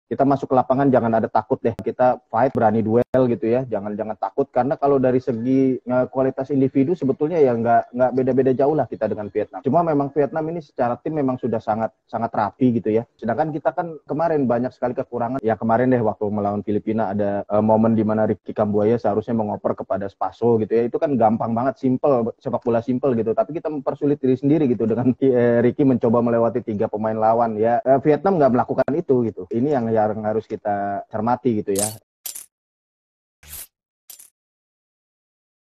SEMIFINAL PIALA AFF, INI YANG PERLU DILAKUKAN TIMNAS INDONESIA MENURUT PENGAMAT!
Pengamat sekaligus jurnalis sepak bola, Dwi Widijatmiko mengatakan, saat masuk ke Lapangan, Timnas Indonesia jangan ada rasa takut, dan harus berani duel.

Menurutnya, jika dilihat dari kualitas individu (pemain) tidak berbeda jauh dengan Vietnam. Timnas Indonesia pun harus bermain dengan sempurna, dan jangan membuat kesalahan.